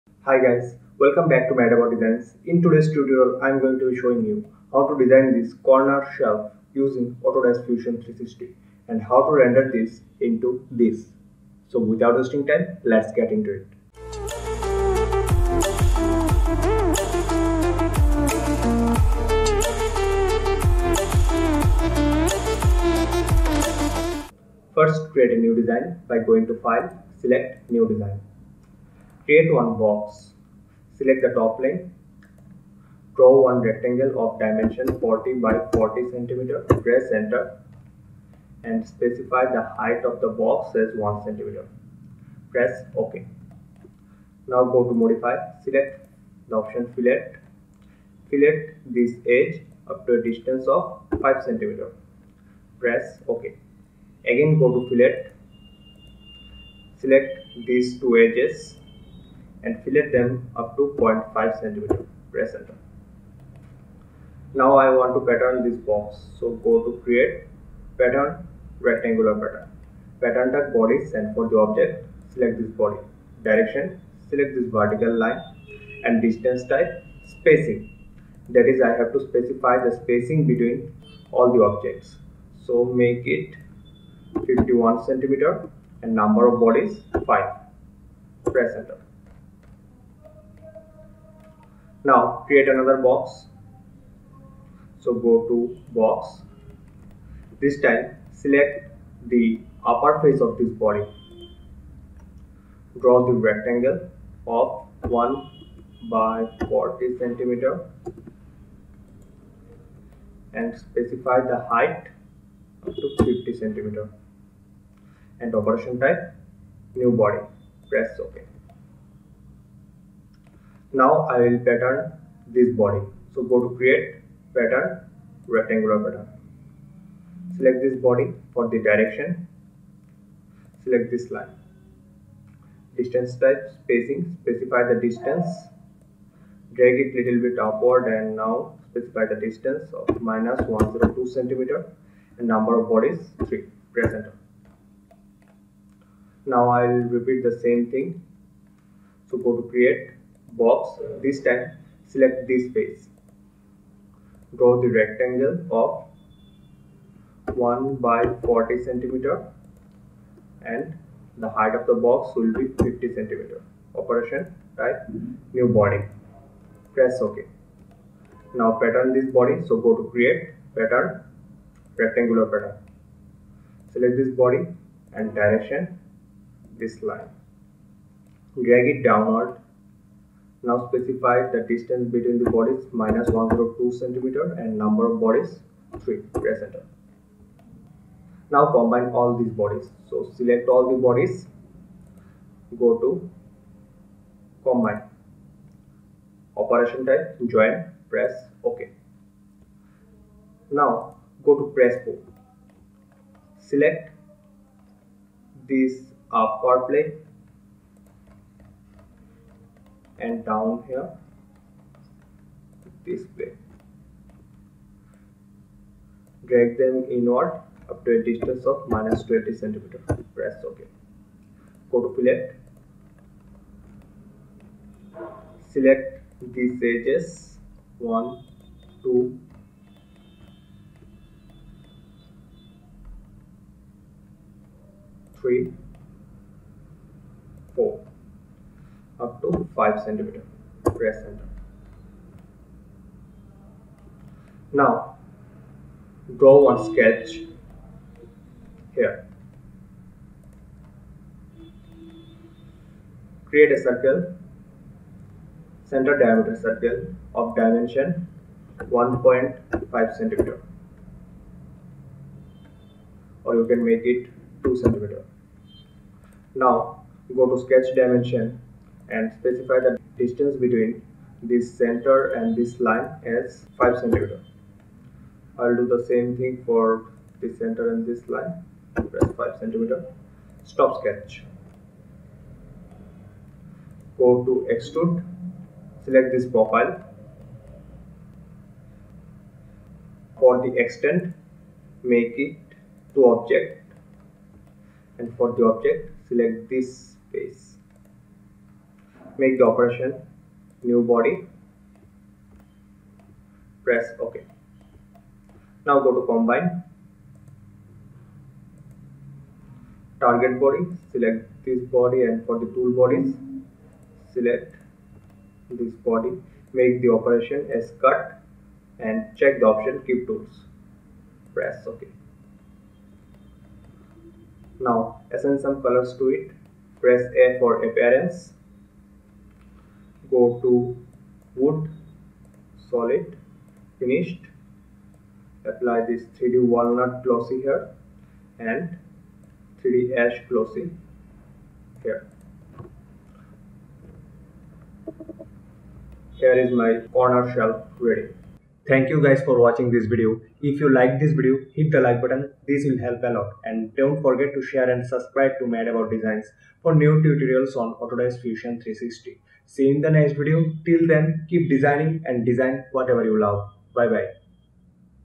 How to design a Wooden Corner Shelf in Autodesk Fusion 360 | Fusion 360 Tutorial
Autodesk Fusion 360 tutorial showing how to design a Corner Shelf.

Tools used in this tutorial are
1. Rectangular Pattern
2. Press Pull
3. Combine

For more designs follow on